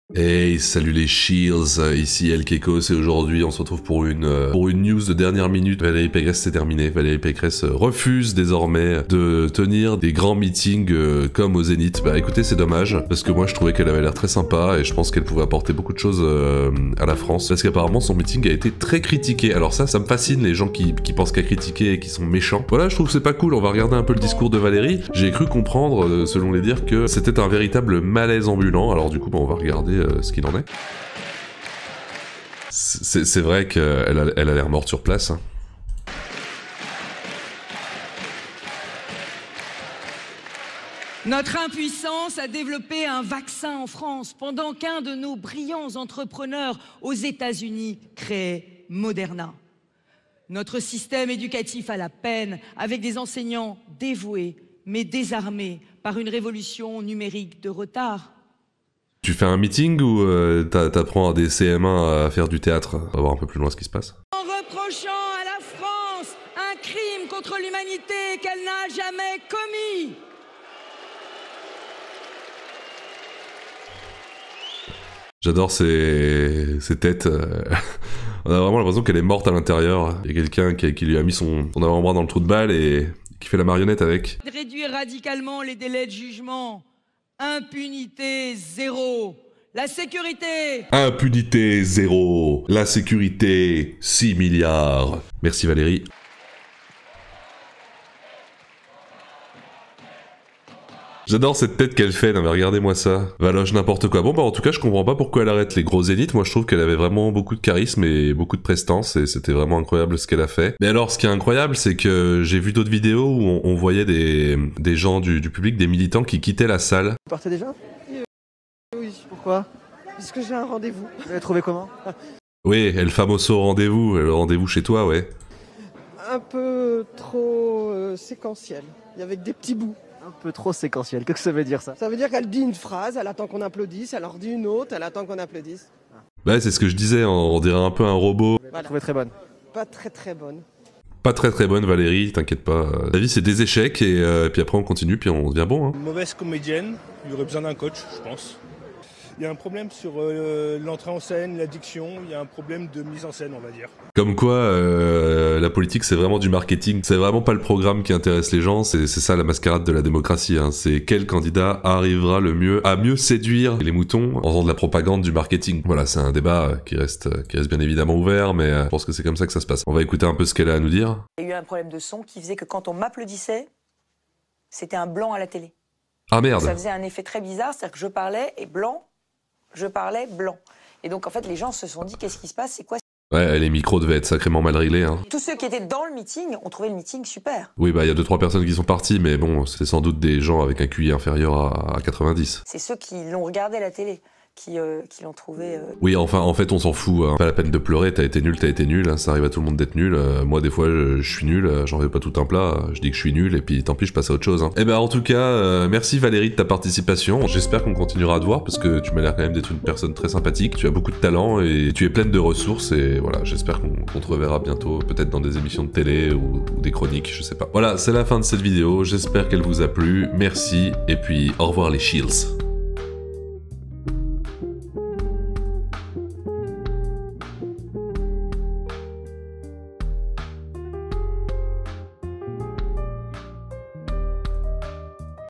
GIGA MALAISE AU MEETING DE VALERIE PECRESSE - LES MILITANTS QUITTENT LA SALLE ET ELLE RAGE QUIT
Valérie Pécresse a tenu un meeting devant 7000 personnes, petit problème, elle était possédée par un robot qui lui faisait faire des têtes bizarres et dire des phrases incohérentes. Résultat, elle a provoqué tellement de malaise (catégorie 4) que les militants ont quitté la salle. Elle accuse un problème de micro, ce qui semble difficile à croire car les robots de 2022 sont tous bien amplifiés.

On pourrait penser qu'elle n'a plus aucune chance face à Macron ou Zemmour, cependant sa nouvelle force de Terminator pourrait l'aider à l'emporter.

La suite au prochain Pécresso-malaise...